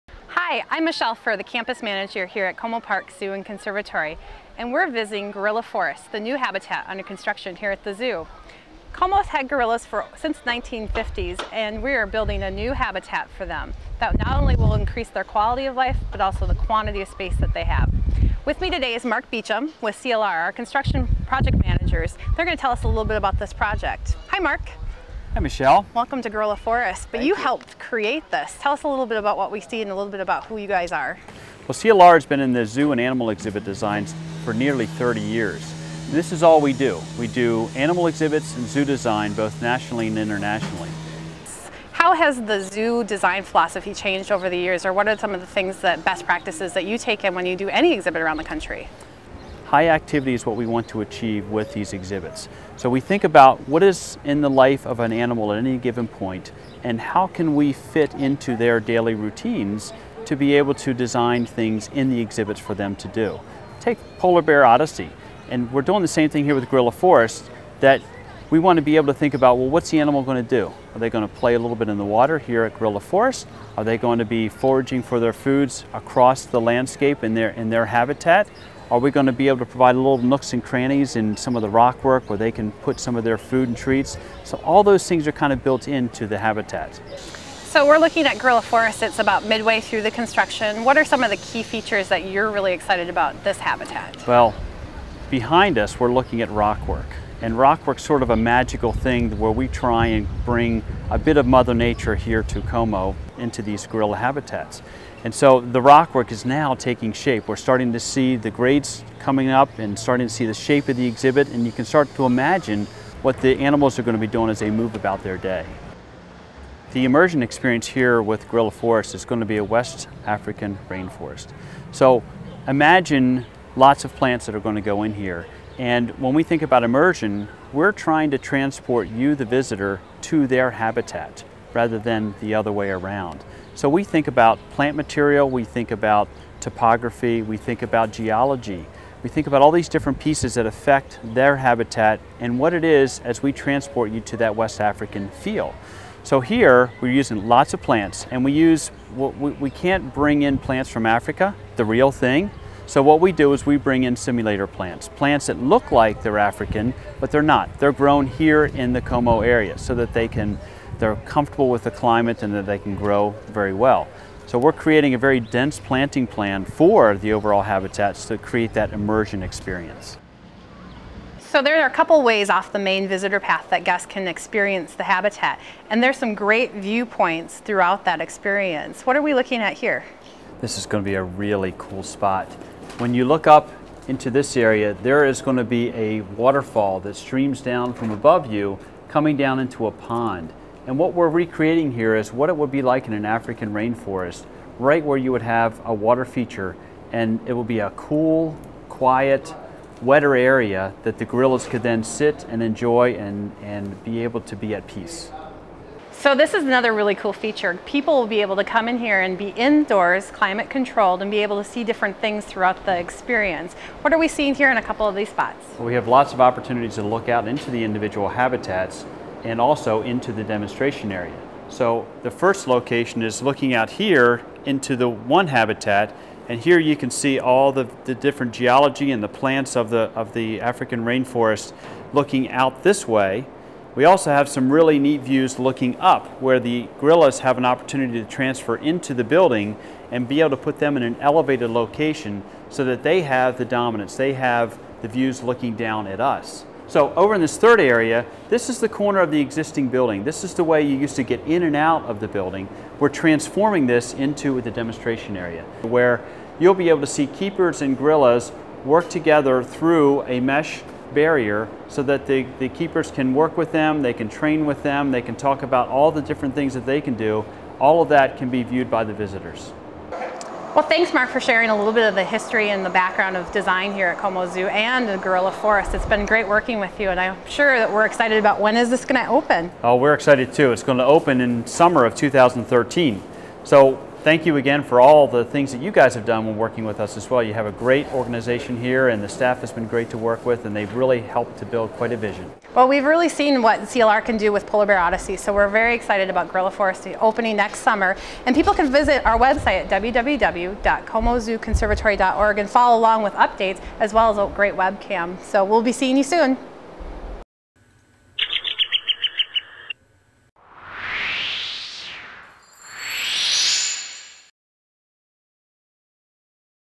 Gorilla Forest Update
Saint Paul's Como Zoo is building a new habitat for its gorillas, scheduled to be open in the summer of 2013! See how the project is going with Como's Michelle Furrer and exhibit designer Mark Beauchamp.